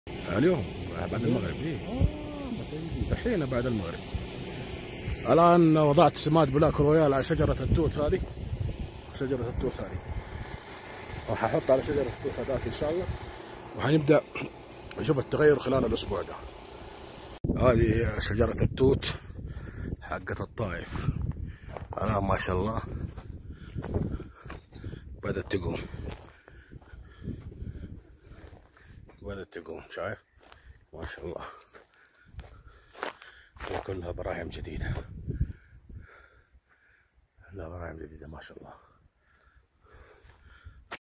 تجربة المؤكسد وبلاك رويال علي توت الطائف
تجربة المؤكسد وبلاك رويال علي توت الطائف تم عرض فيديو سابق والان سوف يتم عرض قبل وبعد بناء علي رغبة الجمهور